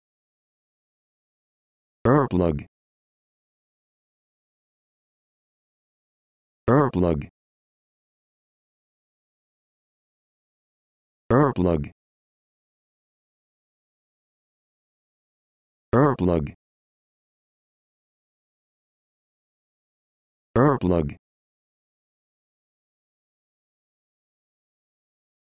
How to Pronounce Earplug
This video shows you how to pronounce Earplug